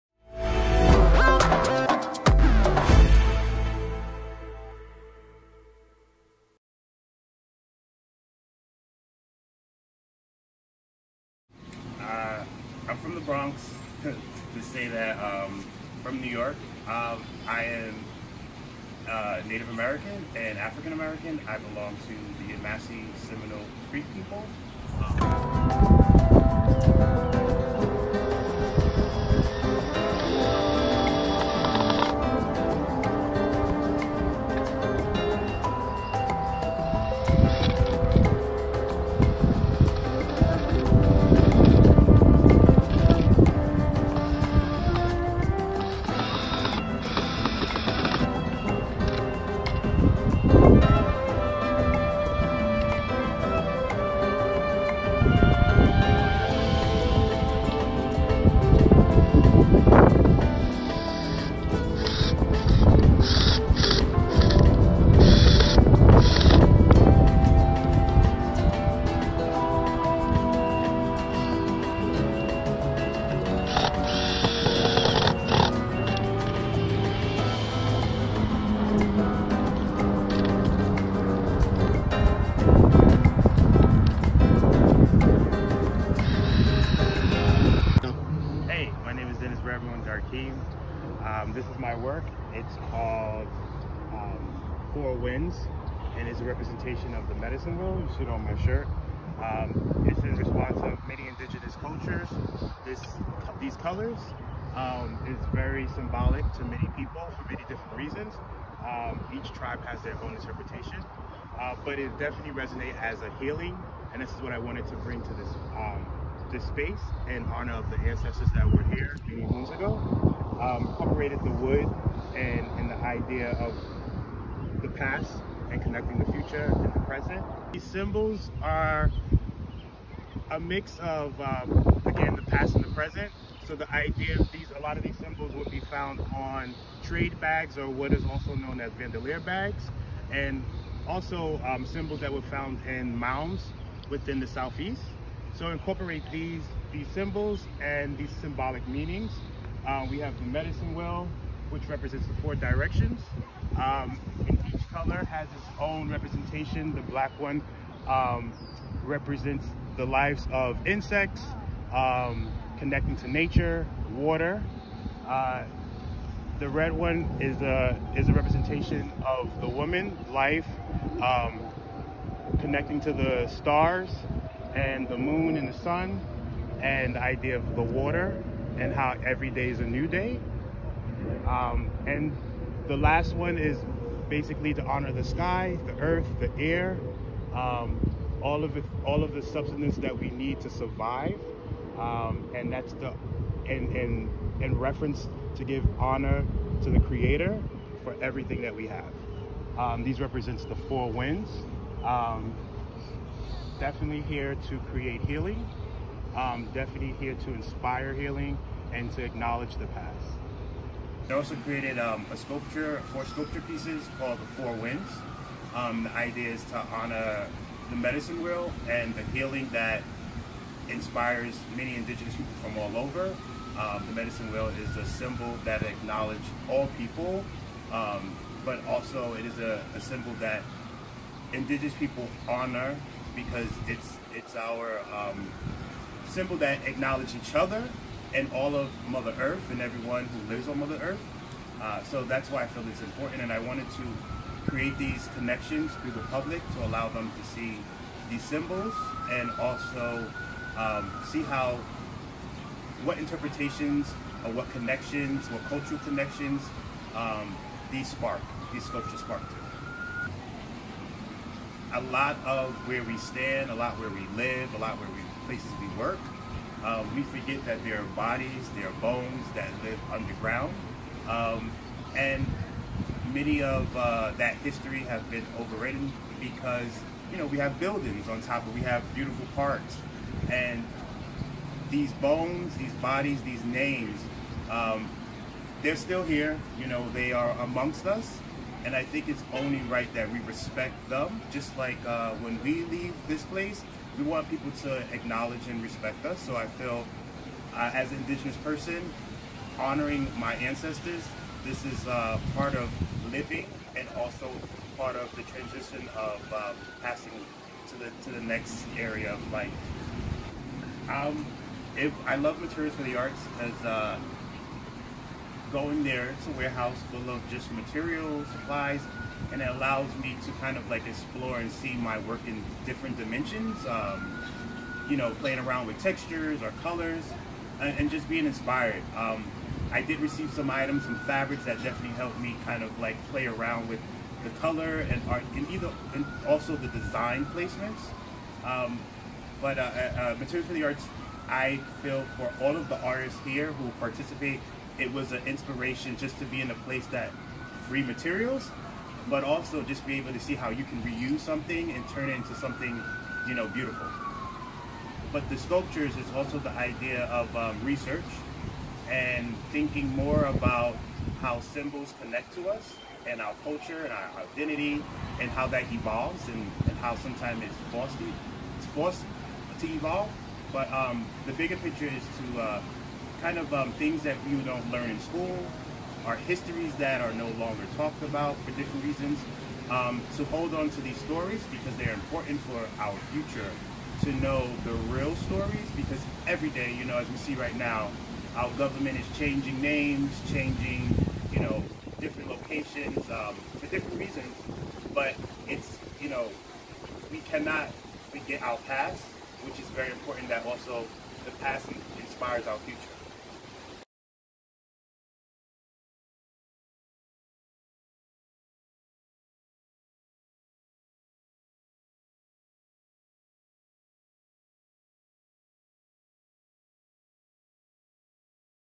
2021 AnkhLave Garden Project Fellow: Dennis Redmoon Darkeem
“Keepers of the Four Winds”
Dennis created these sculptures near the Meadow, Green Trailers at the Queens Botanical Garden.

These four sculptures are painted in the medicine wheel colors White, Red, Black & Yellow. To honor the elements of nature and the colors of mankind. The symbolisms in these sculptures are connected to symbols found in indigenous mounds in Native communities along the East Coast and Central America. These symbols tell stories of what was present at that time.

​BIOGRAPHY:
Dennis RedMoon Darkeem is an artist inspired to create art work based on the familiar objects that he views through his daily travels.  He discovers elements in existing architecture and among everyday items found within the home.  He ultimately set out to express a meaningful story about events in his life and those found with the communities he works with. He utilizes different media in the creation of his work. This allows for great versatility and a rich viewer experience as the eye uncovers the multiple layers that often characterize mixed media art.